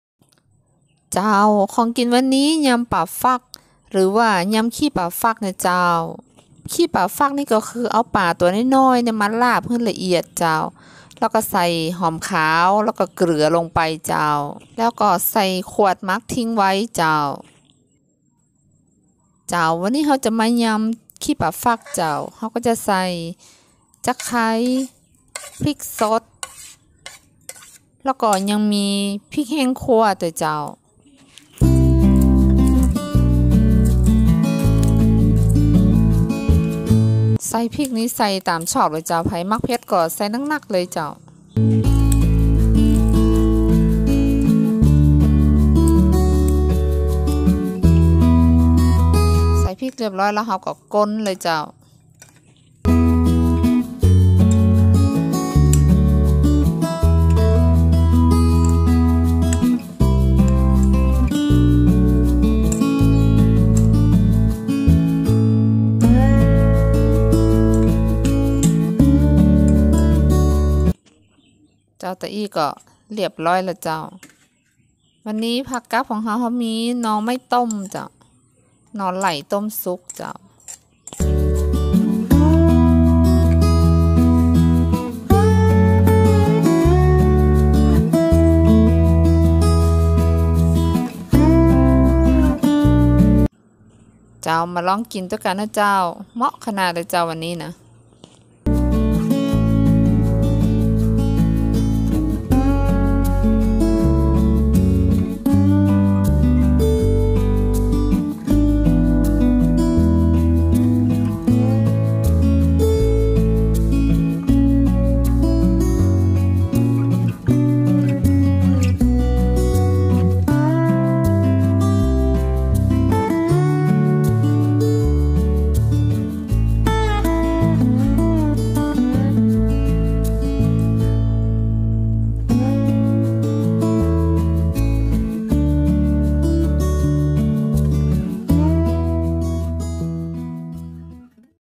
ยำปลาฟัก(ยำขี้ปลาฟัก)อาหารพื้นบ้าน อาหารเหนือ
ปลาฟักคือการเอาปลาตัวเล็กๆมาสับให้ละเอียดใส่เกลือกับกระเทียมแล้วนำใส่ขวดหรือกระปุกหมักไว้ประมาณ3หรือ4คืนก็นำมากินได้แล้วค่ะ